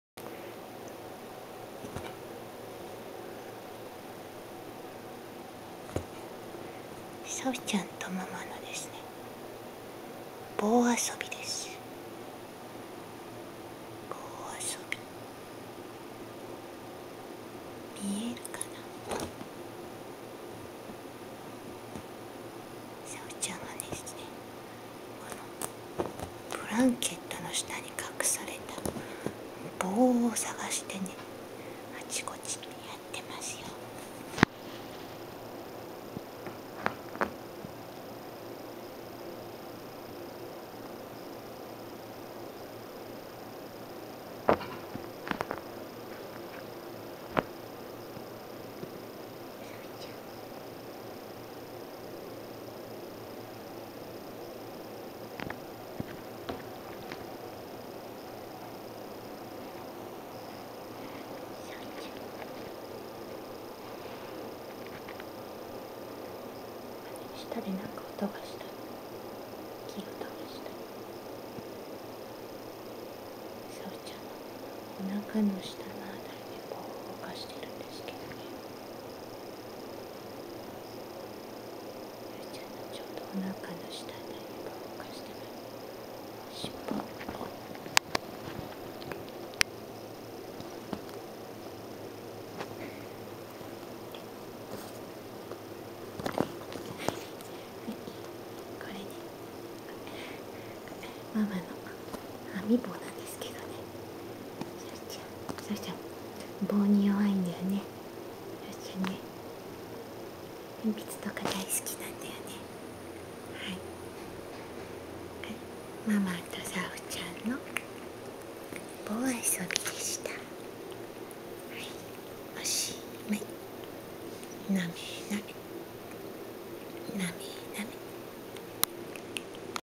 Saf looking for bamboo stick 棒遊び中のサフ
Saf is looking for bamboo stick which Mama is hiding and moving under blanket  棒に弱いんだニャー　編棒、どこかニャー？